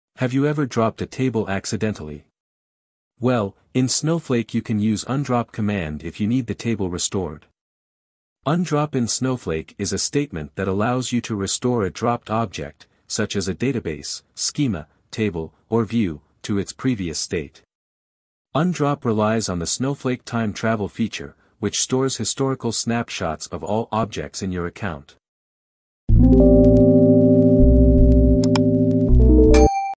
UNDROP Command in Snowflake - Short
Have you ever dropped a table accidentally. Well, in Snowflake you can use UNDROP command if you need the table restored.

UNDROP in Snowflake is a statement that allows you to restore a dropped object, such as a database, schema, table, or view, to its previous state. UNDROP relies on the Snowflake Time Travel feature, which stores historical snapshots of all objects in your account.